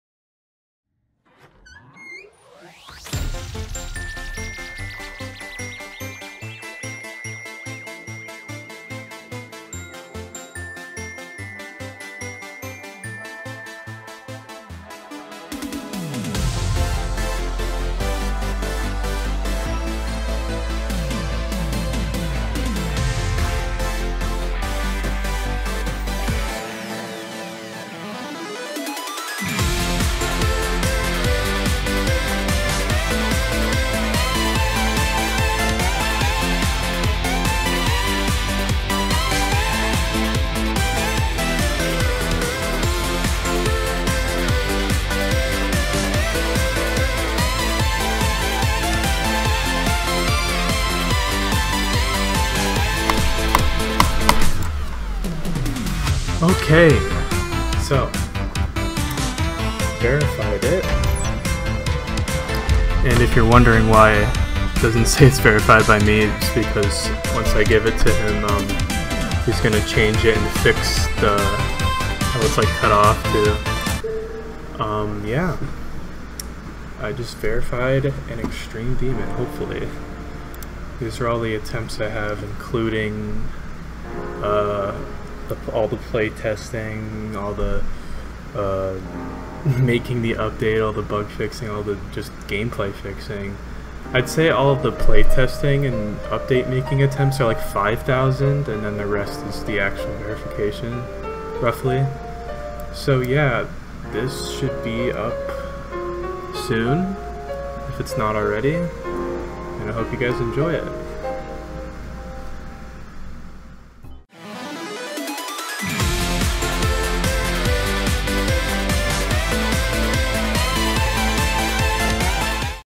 [UPDATE VERIFIED!] "Effect" by MrSupremeGD (Insane Demon)
ID: 67188780

Thank you so SO much to MrSupreme for giving me the opportunity to update and verify this fantastic level! This level needed a TON of changes from its original version. The OG version was filled with choke points, bugs, literal INVISIBLE STRUCTURES AND SPIKES (not sure how that got past verification), and frankly unfun gameplay. However, thanks to me and my wonderful set of playtesters (skzyl, [redacted], Kutsu, Zain, and PrankOrigami) this level is a million times more fun, and definitely the most fun extreme I've beaten. Hopefully it is still extreme, it is overall easier than the original version, but that's only because it's more balanced and less unfair. Also as I said in the video the endscreen with be fixed in the actual level. I really hope you all enjoy this, I had a ton of fun making this update and verifying, and hopefully I get another verification project sometime in the future!

Btw thanks to skzyl for the thumbnail! He's an insanely talented player so go gas him up! His channel

Attempts: 6137 (including ~5k playtesting and update making attempts)

Monitor Refresh Rate: 144hz (288fps bypass)
Mouse: Logitech G502
Microphone: Blue Snowball iCE